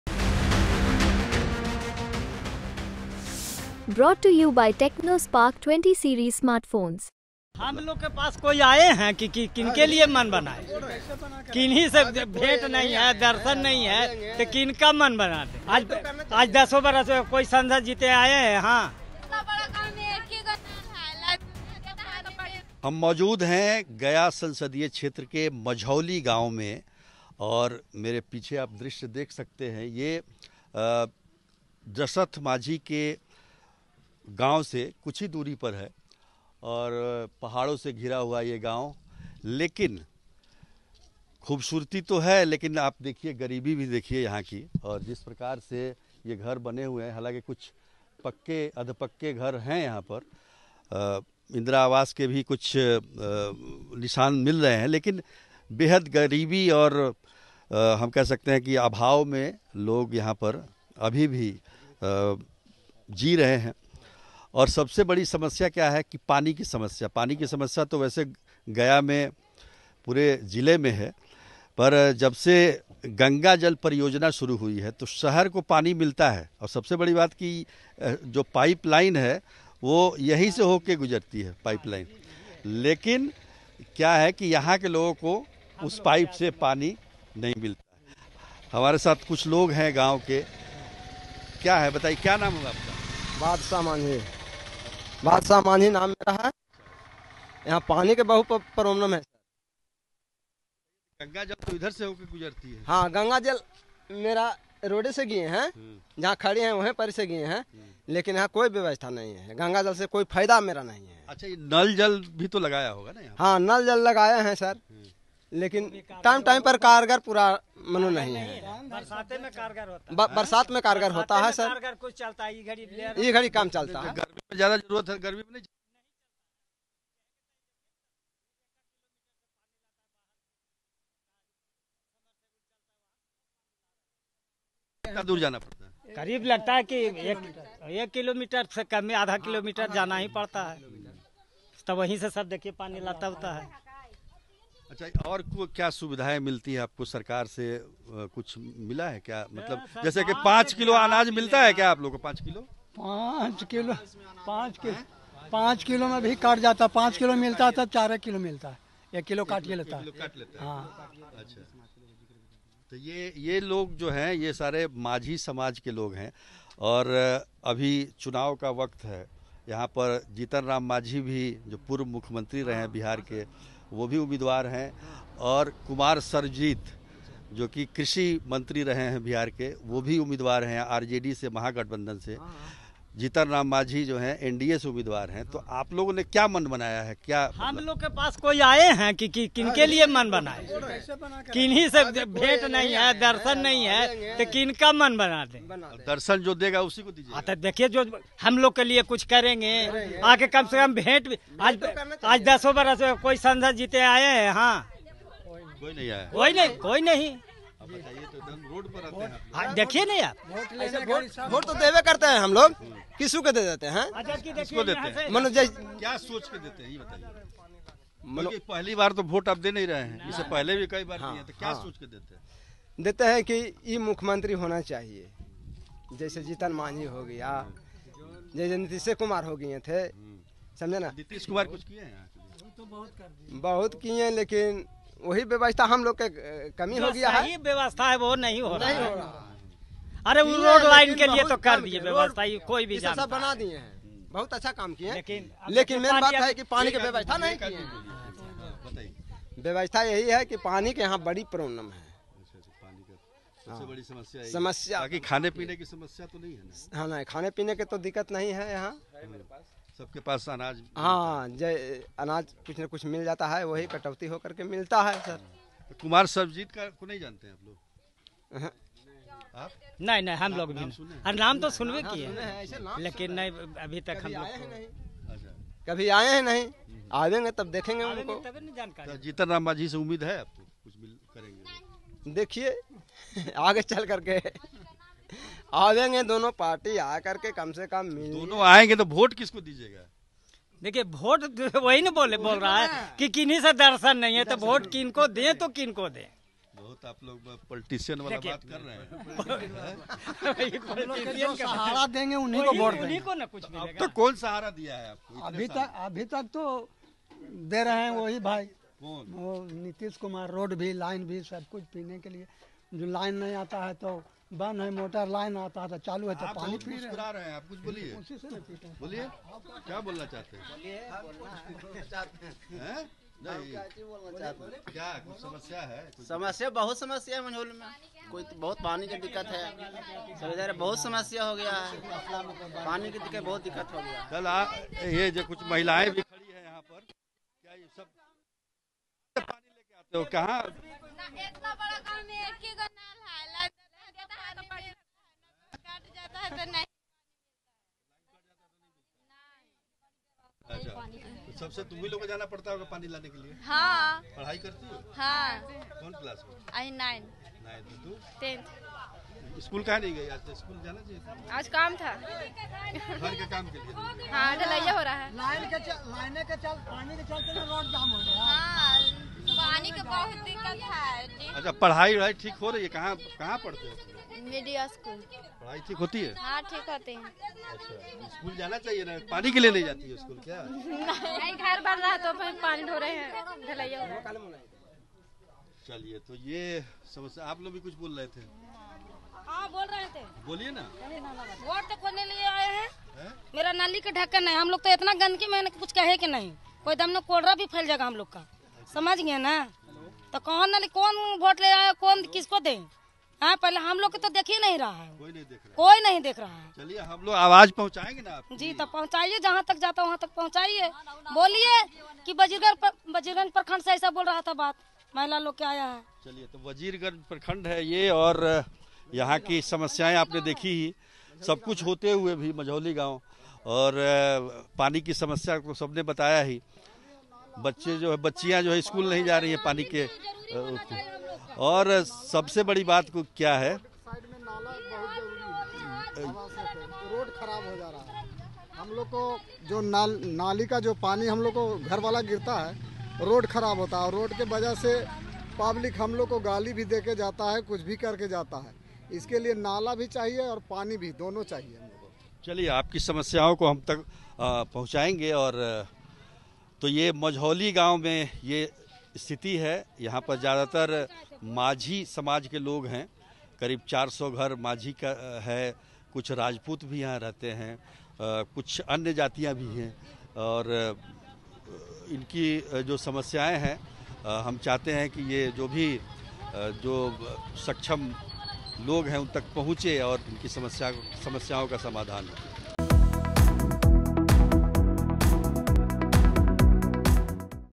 Dashrath Manjhi के गांव वाले बूंद-बूंद को तरसे, बोले जात नहीं पानी पर देंगे वोट | Bihar Tak
बिहार तक चुनावी यात्रा के दौरान गया में दशरथ मांझी के गांव पहुंचा जहां लोगों की सबसे बड़ी समस्या पानी है, जनता का साफ कहना है जाति नहीं पानी पर वोट देंगे.